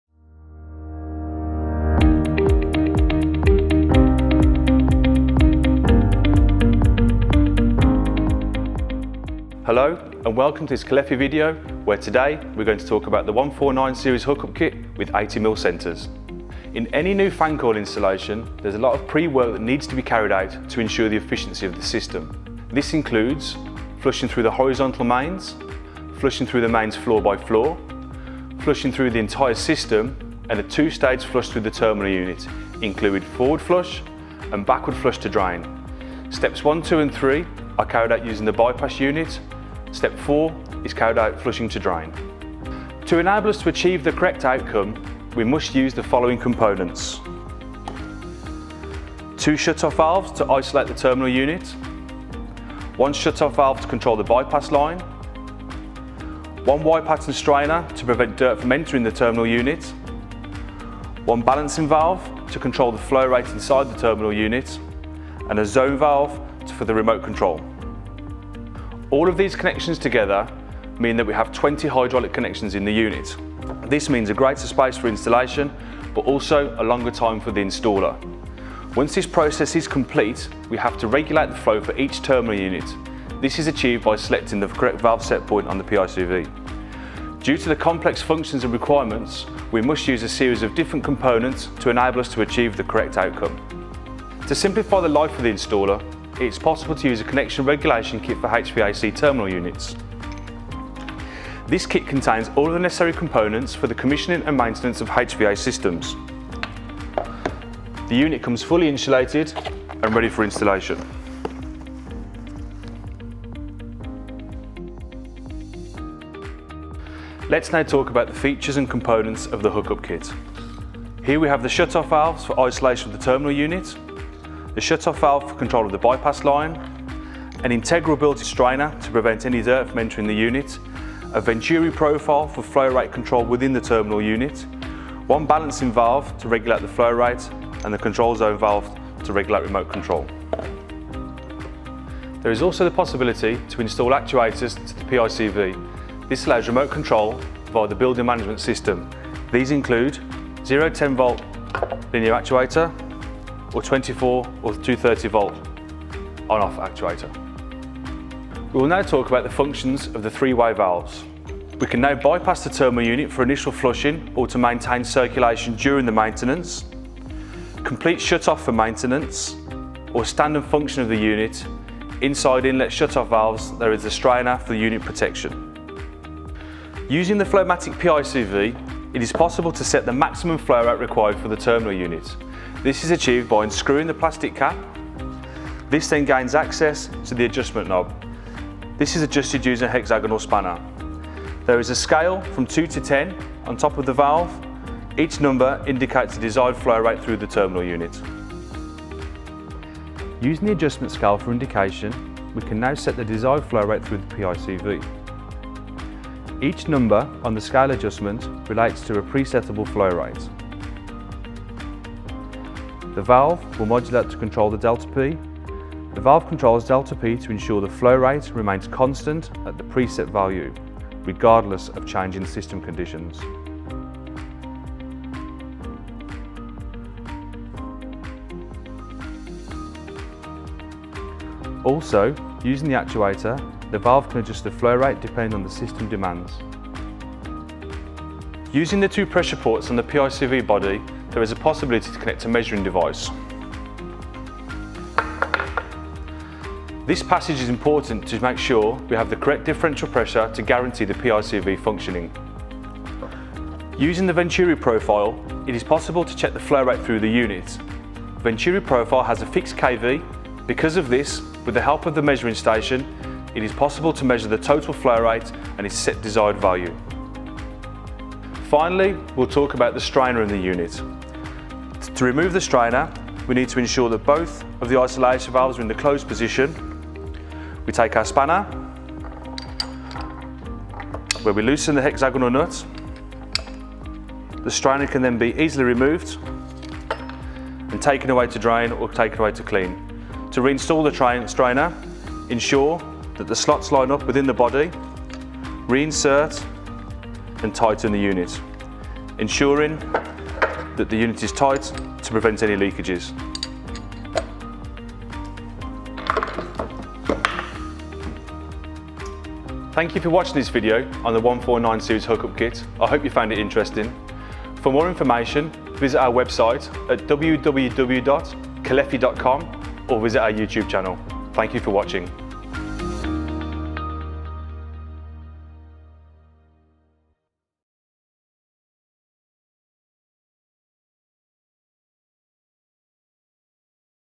How does a connection and regulation kit for HVAC terminal units work?
The pre-assembled group for terminal units is compact and able to
shut-off, adjust and filter the secondary circuit of the terminal unit.
It also allows to perform maintenance and setting operations of the
system.
It allows the connection of fan coils, cold beams or ceiling conditioning
systems with the main distribution network.
Complete with insulation suitable for both heating and cooling.
Available with Venturi device for flow rate measurement.